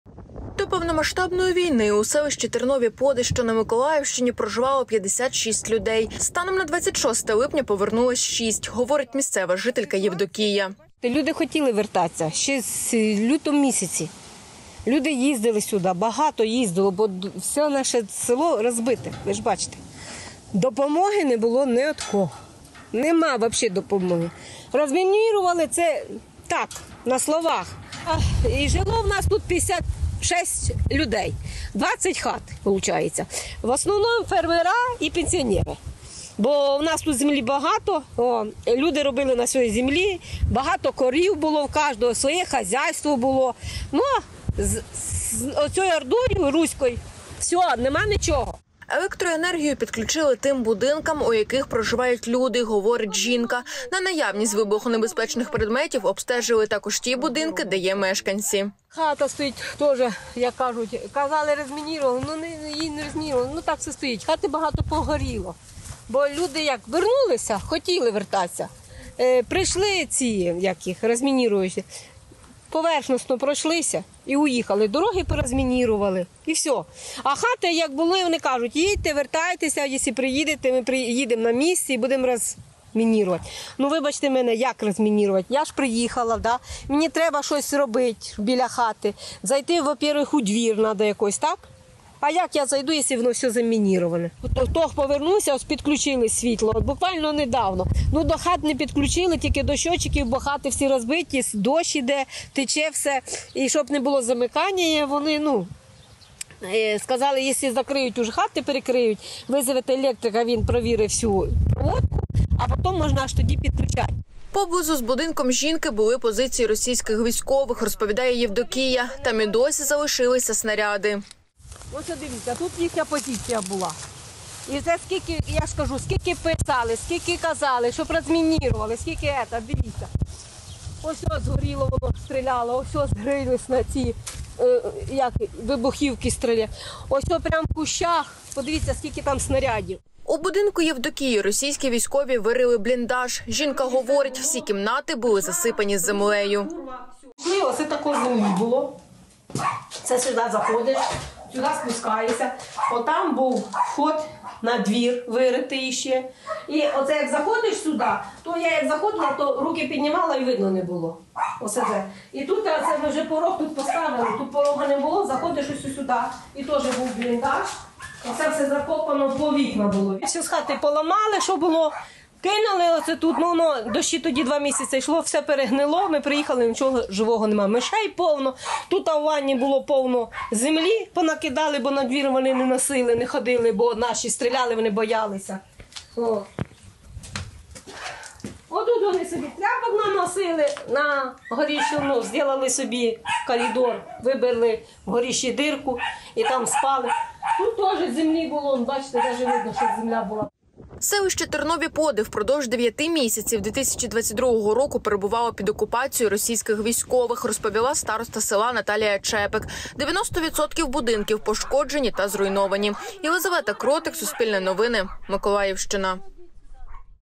Жителька села Тернові Поди повернулася до зруйнованого будинку
Жителька деокупованого села Тернові Поди, що на Миколаївщині, два місяці тому повернулась до свого будинку, в якому впродовж 9 місяців проживали російські військові. Євдокія разом із чоловіком самостійно відновлюють своє помешкання.